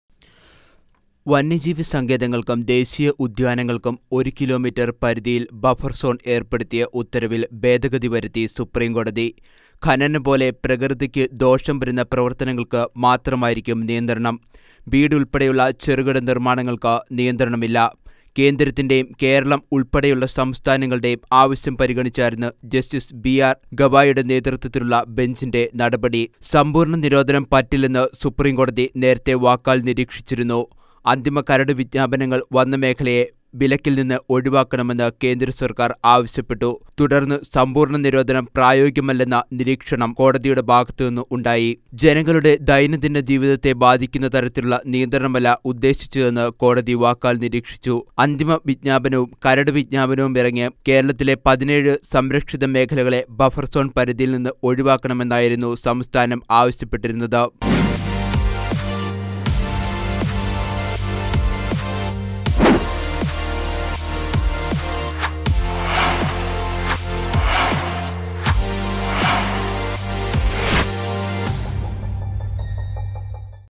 കത്തോലിക്ക സഭ ഉള്‍പ്പെടെയുള്ളവരുടെ പോരാട്ടം ഫലം കണ്ടു .ബഫര്‍ സോണില്‍ നിര്‍ണ്ണായക വിധി BUFFER ZONE
കത്തോലിക്ക  സഭ ഉള്‍പ്പെടെയുള്ളവരുടെ പോരാട്ടം ഫലം കണ്ടു  .ബഫര്‍ സോണില്‍ നിര്‍ണ്ണായക വിധി
BUFFER ZONE SUPREME COURT VERDICT LATEST

Idukki Vision  Channel No:51
Bhoomika :52
Malanad Vision  :56